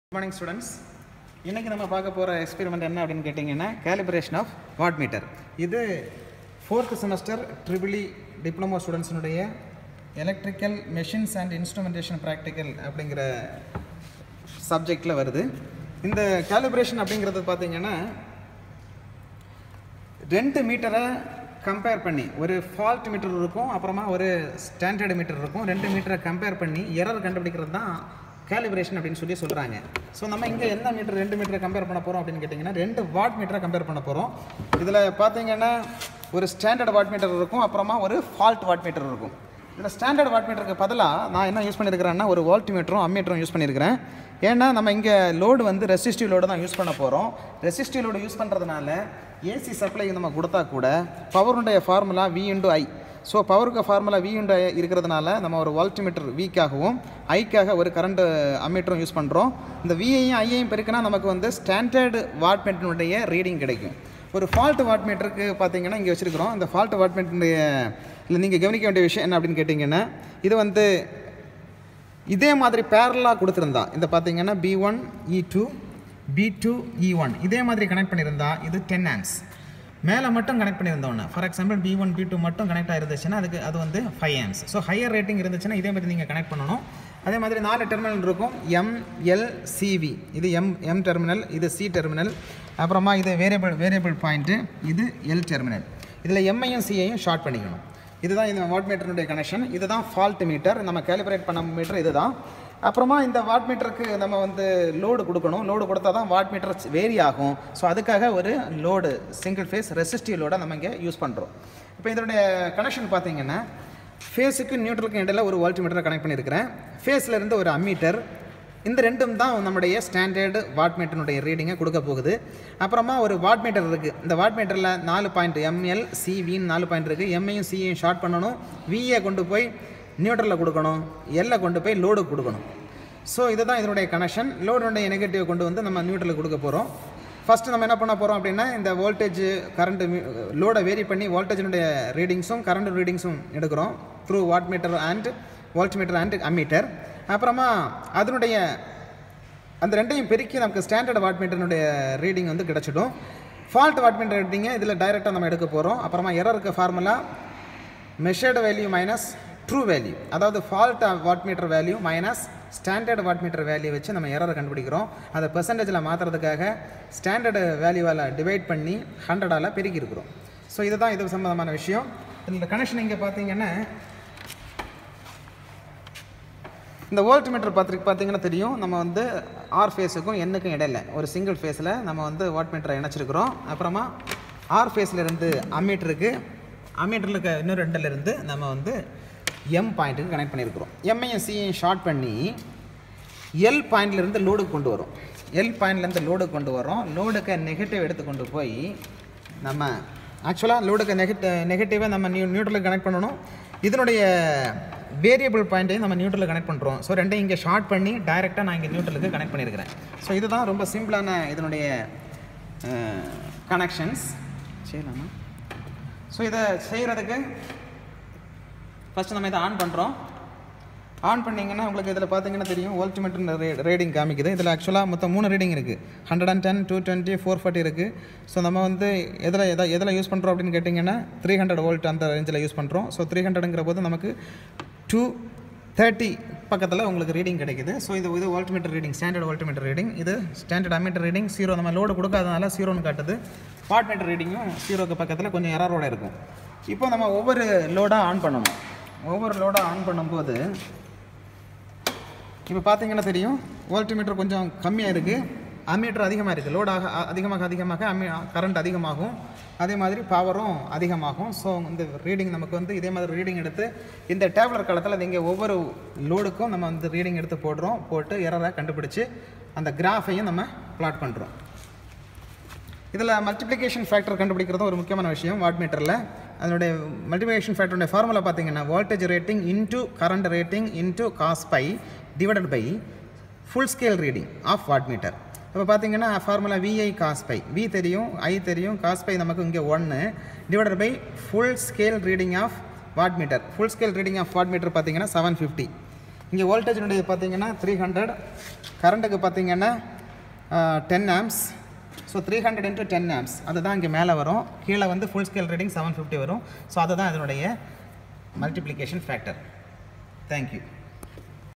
Calibration of wattmeter in Tamil (DEEE-IV SEM EMIP)
Electrical machines and instrumentation practical for fourth semester Diploma EEE Students